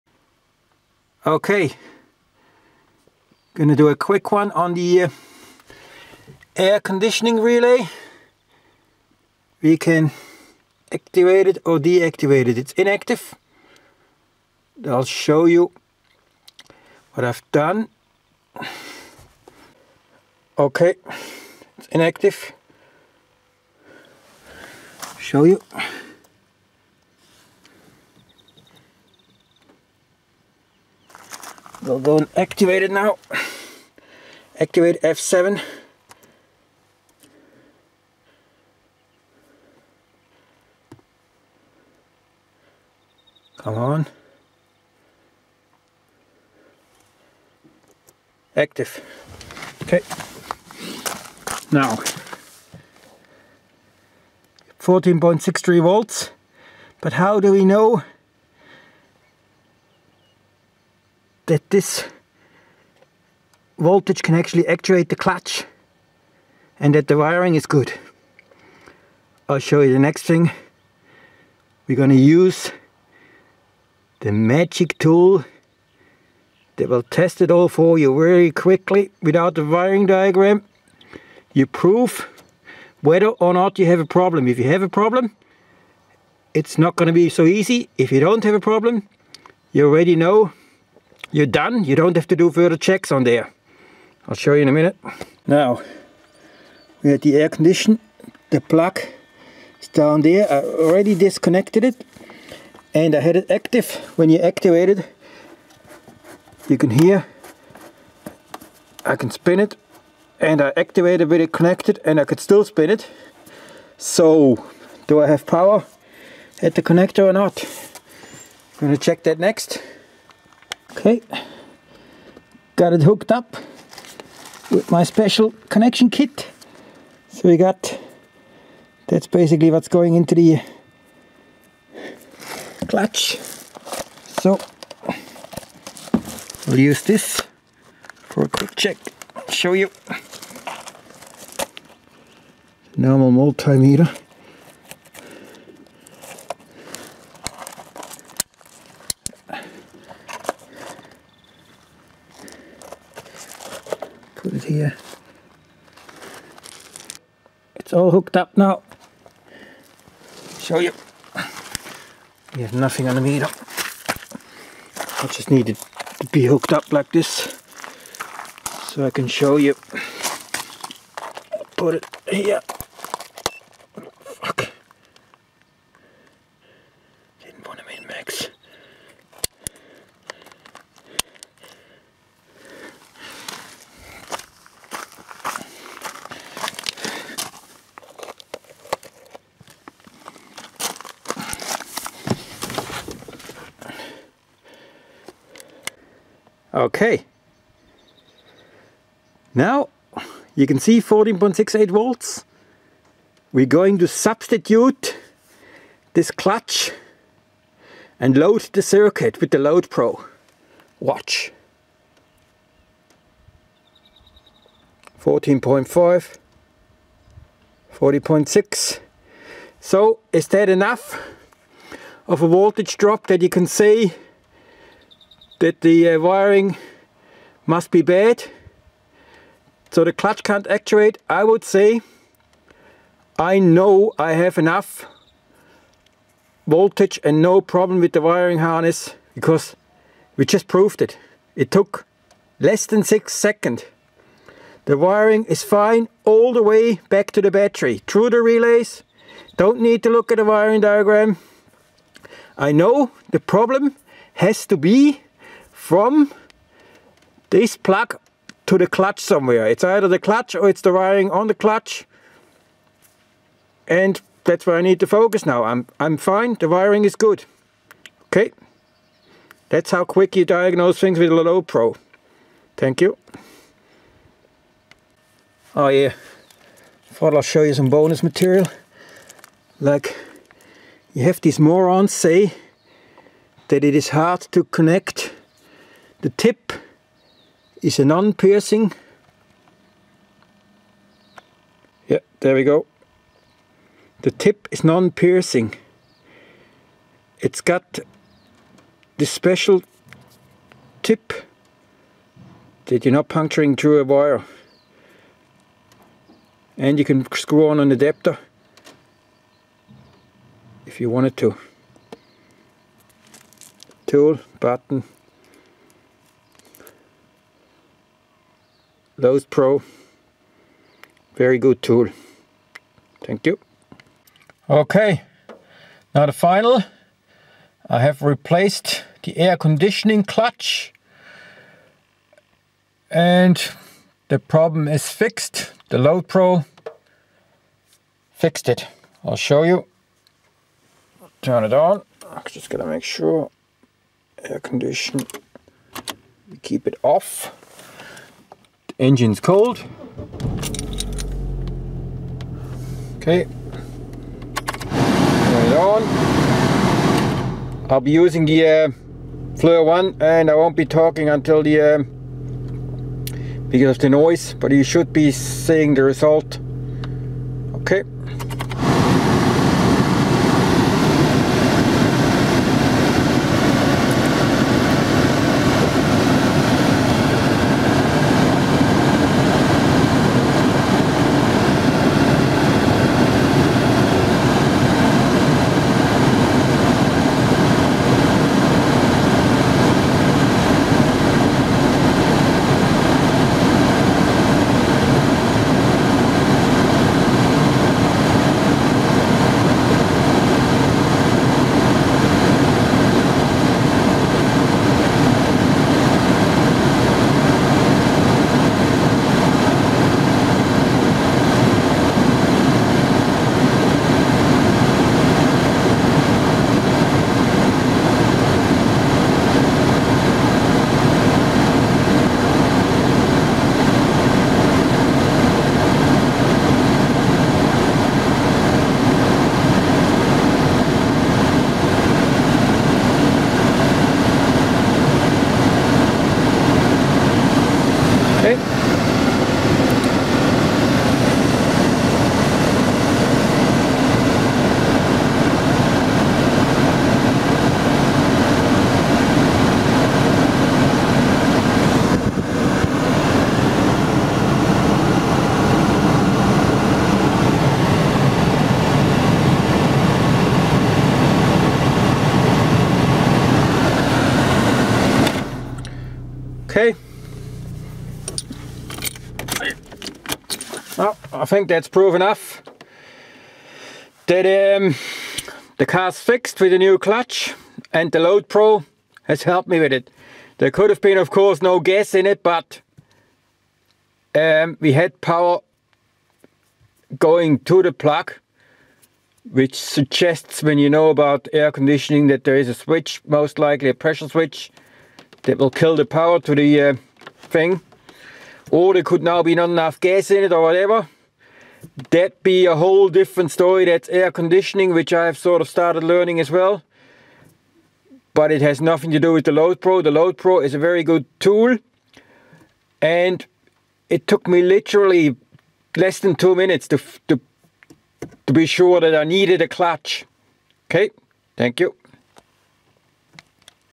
Loadpro Tool in action! GM-Astra airconditioning
This  video took way longer then the Actuarl test with the loadpro. With the loadpro you use  Both tips in the connector push button and your  done  or you have a problem...
it gives a instant evaluation if or not you have a voltage drop .Problem in the wiring  harness to The tested item?.
Especially good to test for bad  grounds on tail light  fuelpumps Anything that has  a non puls-with modulated  power and  ground.

Making videos about this tool is quite difficult because of the minimal time you take to actuarly use  this tool.
The argument that you can use a test light is flawed  in a  way that you dont!! know the actuarl voltage you drop , as where the loadpro shows the drop in voltage on a  volt meter instantly or Not= No problem. 600 page  flow chart to test  wiring harness or  2 minutes  with a loadpro i know what i be doing ... The Loadpro every  time
Thank you Mr  Dan sullivan for a  awsome quick test tool.
This test to conclude what you can do in 6 secounds will take you at least 10 to 15 minutes  by other means for the same conclusive result to be sure that you have no voltage drop on positive or negative side of a  Circuit.
if  you mesure  the voltage with multimeter  you can assume you have no voltage drop, if you use a testlight and it lights you can assume you have no voltage drop.
"Assumption is the mother of all fuckups"
Dont assume! Know  stuff. = in this case  "loadpro and  you" have the Proof.